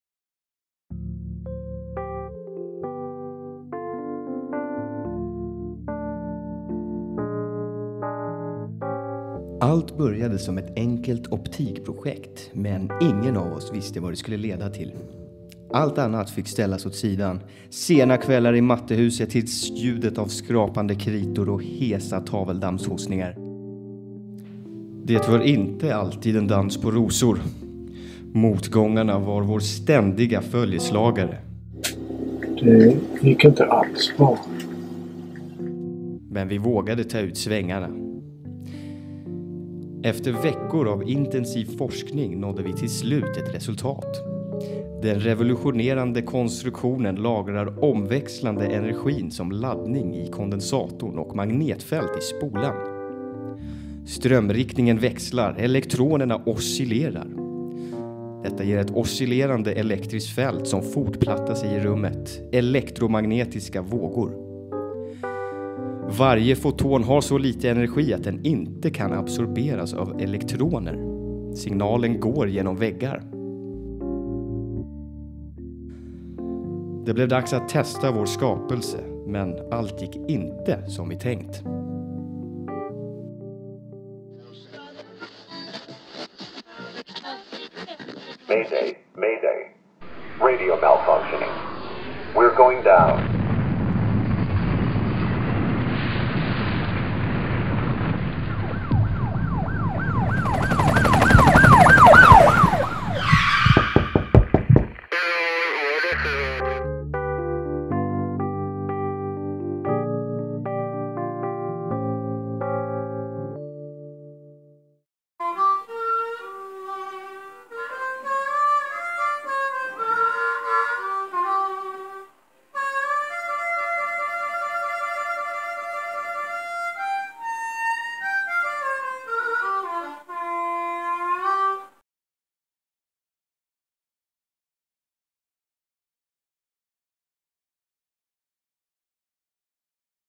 Radio - Ett lite väl vågat Projekt
En film av Carl Henrik Dahmén, Frej Berglind, Johannes Kasimir, Samuel Golobov m. fl.
Denna film gjordes i samband med kursen Våglära och Optik på LTH.